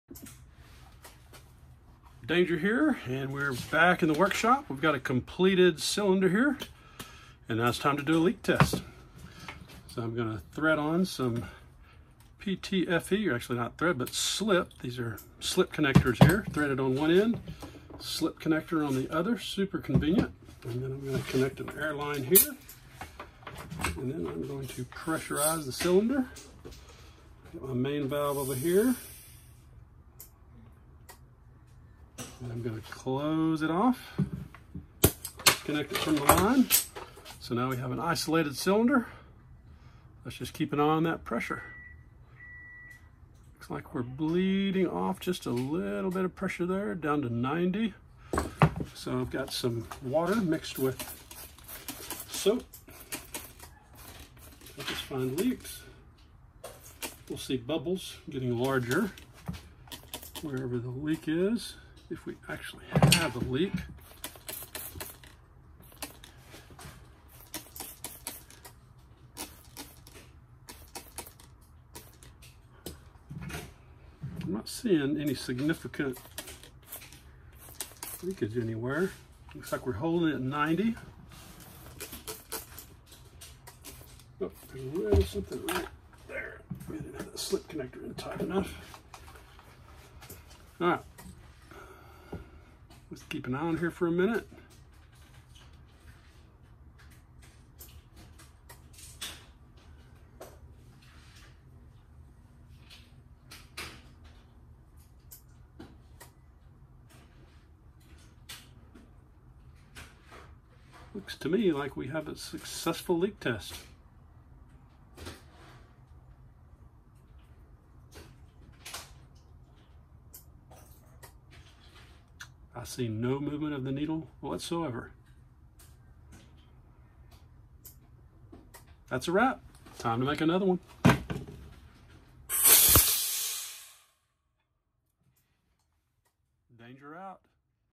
OxyCon 2020 #3 Leak Test
This is one of a series of videos showing the construction of the OxyCon 2020 oxygen concentrator built by Robert Danger Byrd with the engineering support of Ed Mouselli, Programming support of James Zapico and the leadership of Maher Doudi. This is a project of SheildMission.com with a goal to improve the survival rates of COVID-19